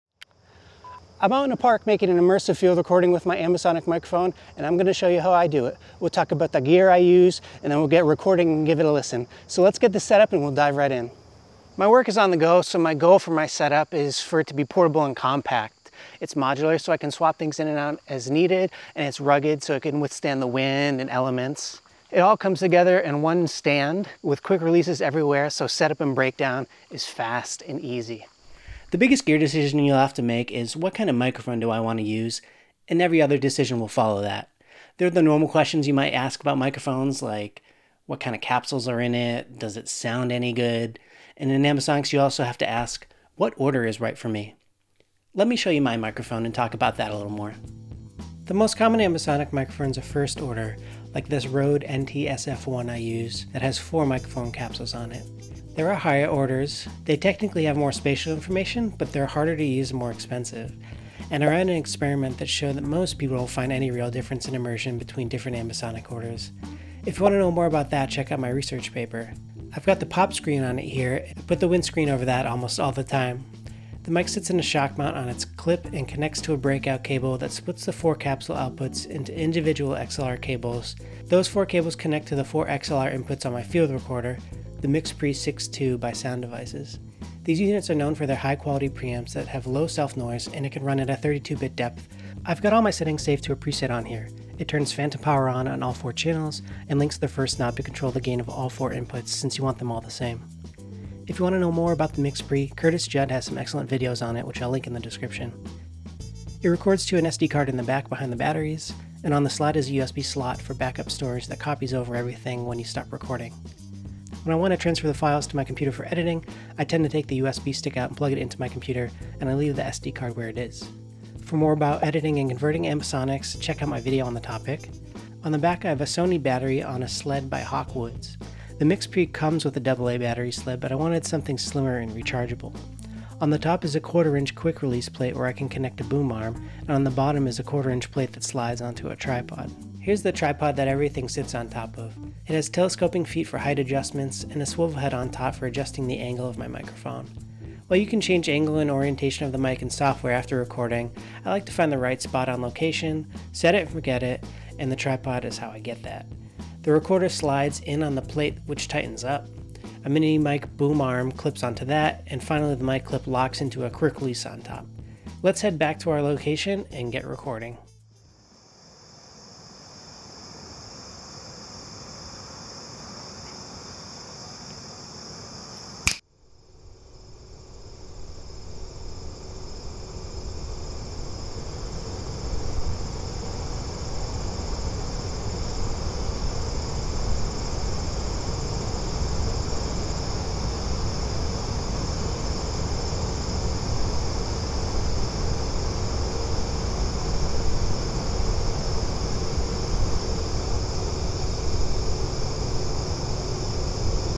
How to make immersive field recordings with ambisonics
Learn how to make field recordings with ambisonics to create immersive nature recordings, using gear and techniques I validated with a research experiment testing different ambisonic techniques.

If you're curious, here's the gear used in this video:
Sound Devices MixPre 6-ii
Rode NTSF-1
Hawk-Woods SD-2 Sound Devices MDV Battery Adapter
Genaray 2 x NP-F770 4400mAh Batteries & 2 x Compact Chargers Kit
PNY 64GB Elite-X Fit USB 3.1 Flash Drive
SanDisk 64GB Extreme SDXC UHS-I Memory Card
SMALLRIG Quick Release Plate Drop-in HawkLock Mini Quick Release Camera Mount Adapter with 1/4" Screw
MeFOTO RoadTrip Aluminum Travel Tripod Kit
Rode Soundfield plugin
Reaper
DearVR ambi micro

You can also find me on my website intunewithian.com and on instagram @intunewithian.

Chapters
00:00 Intro
00:11 Gear philosophy
00:28 Microphone selection
01:01 Ambisonic order experiment
01:23 Field recorder
02:21 Tripod and mounts
02:46 Recording
02:56 Listening